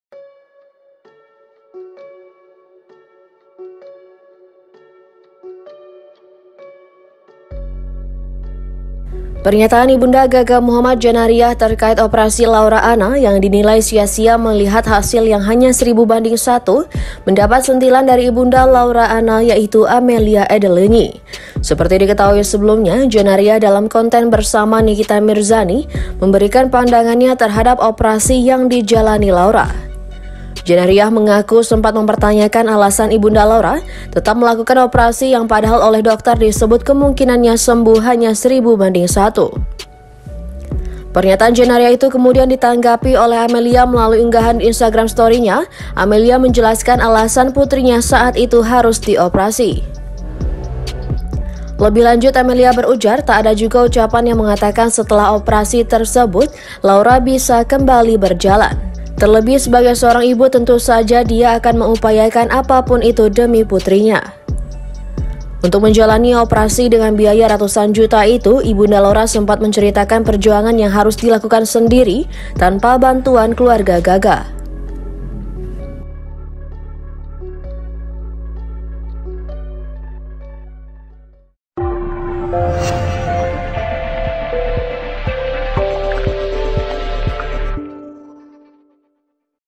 HASIL OPERASI DIPERTANYAKAN IBU GAGA MUHAMMAD, IBU LAURA ANNA: ANAK SAYA KESAKITAN
TRIBUNJAMBI.COM-Pernyataan ibunda Gaga Muhammad, Janariyah terkait operasi Laura Anna yang dinilai sia-sia melihat hasil yang hanya 1.000: 1, mendapat sentilan dari ibunda Laura Anna, Ameilia Edelenyi.

Seperti diketahui sebelumnya, Janariyah dalam konten bersama Nikita Mirzani memberikan pandangannya terhadap operasi yang dijalani Laura. Janariyah mengaku sempat mempertanyakan alasan ibunda Laura tetap melakukan operasi yang padahal oleh dokter disebut kemungkinannya sembuh hanya1.000: 1.

Pernyataan Janariyah itu kemudian ditanggapi oleh Ameilia melalui unggahannya di Instagram Story. Ameilia menjelaskan alasan putrinya saat itu harus dioperasi.